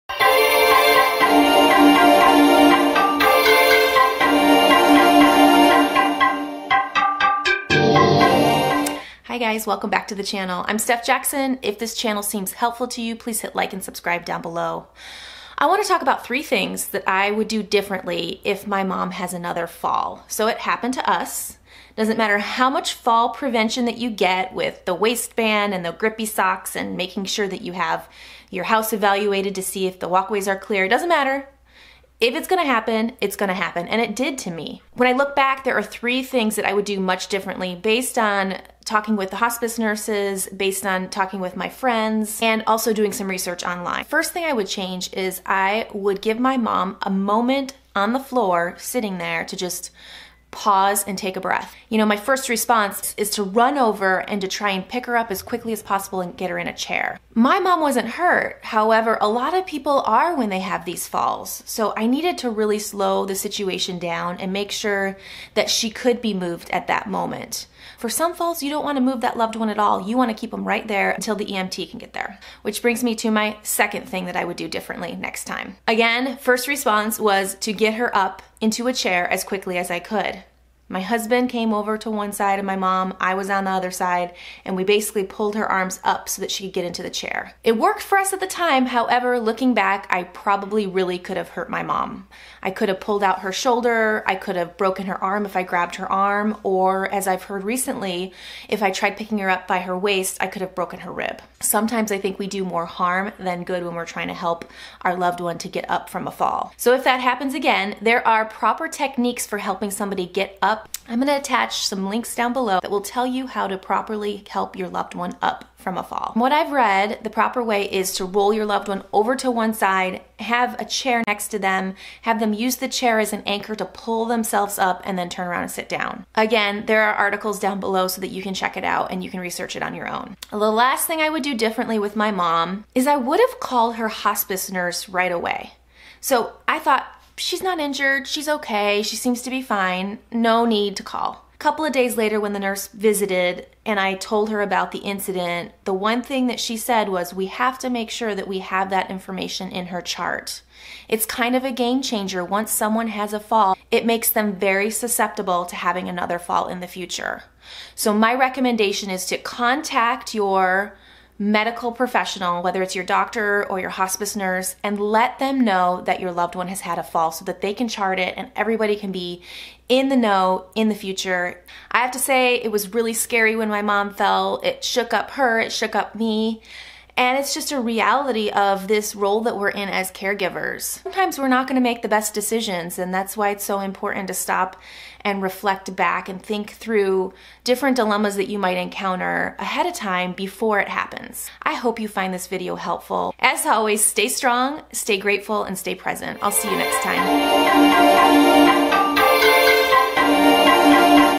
What Do I Do When She Falls?
Three things I will do differently if my mom falls again. No joke, her fall happened fast and it took me by surprise. Think it through before it happens to you.